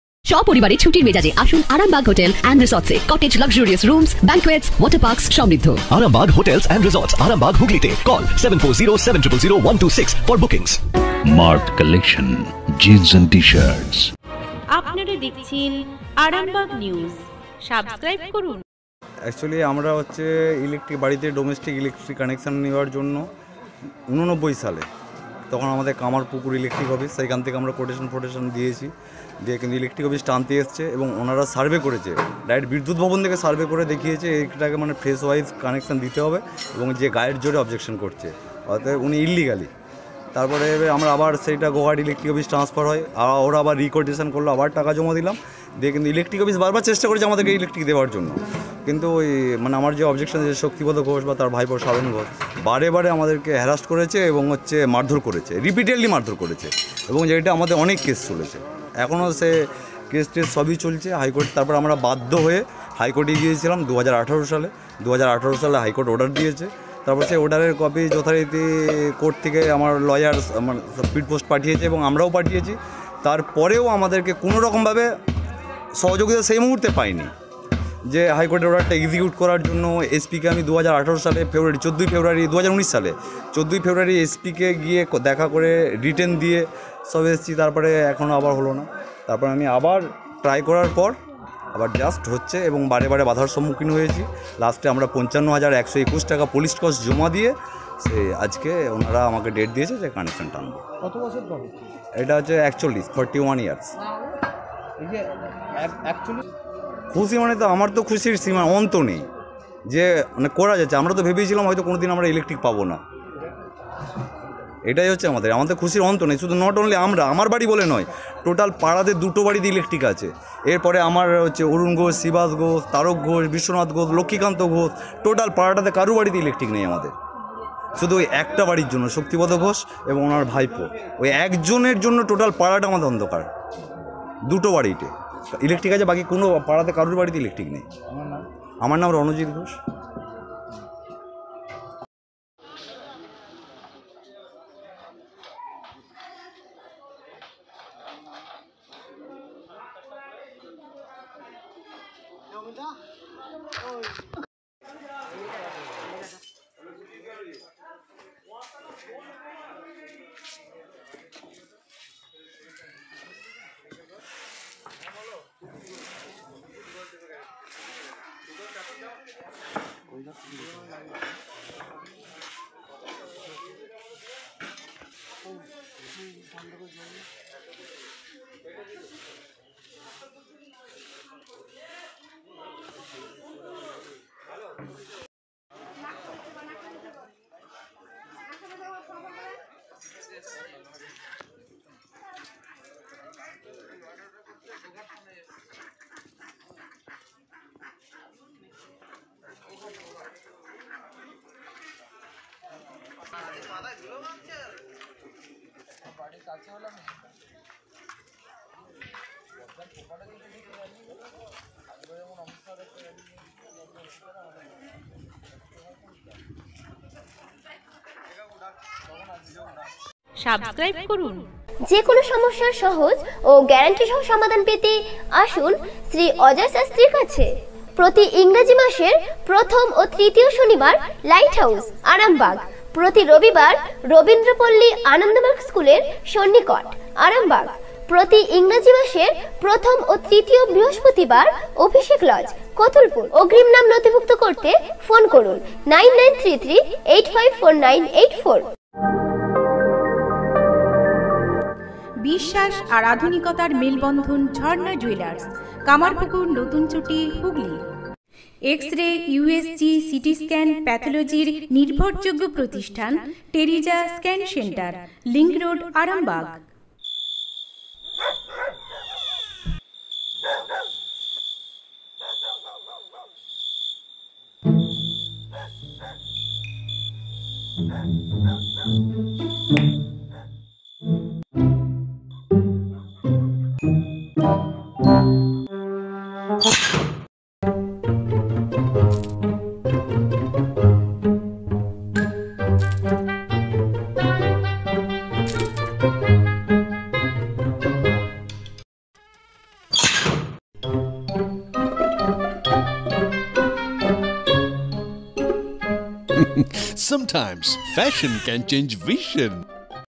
চার দশক পরে কোর্টে জিতে পুলিশ পাহাড়ায় বিদ্যুৎ কানেকশন পেল গোঘাটের শুনিয়ার এক ঘোষ পরিবার।
আরামবাগ নিউজ ফোন/হোয়াটসঅ্যাপ - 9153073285 /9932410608(এডিটর)।